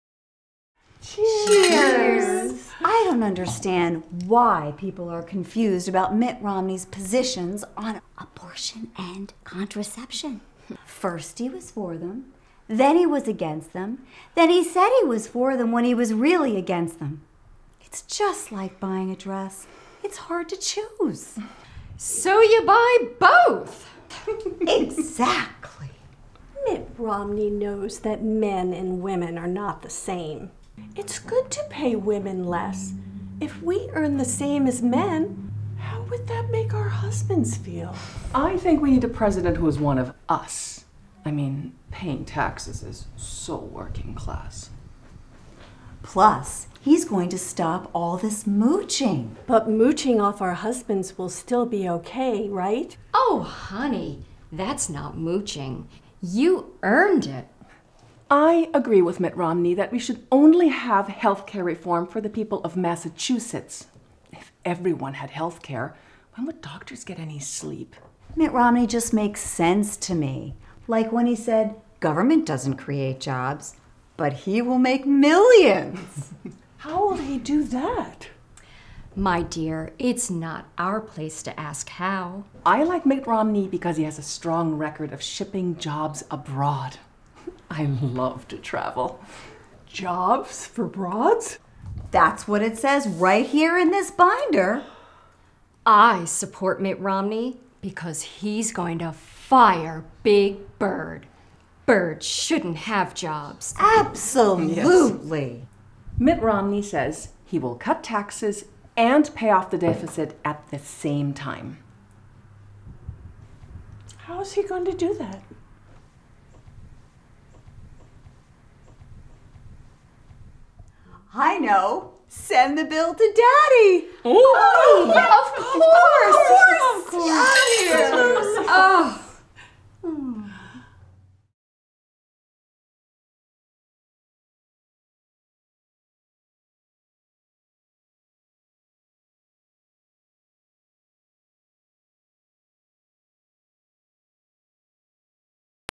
Stepford Wives for Romney
Confused about all the different positions Mitt Romney has taken on the issues? Let the Stepford Wives explain it to you...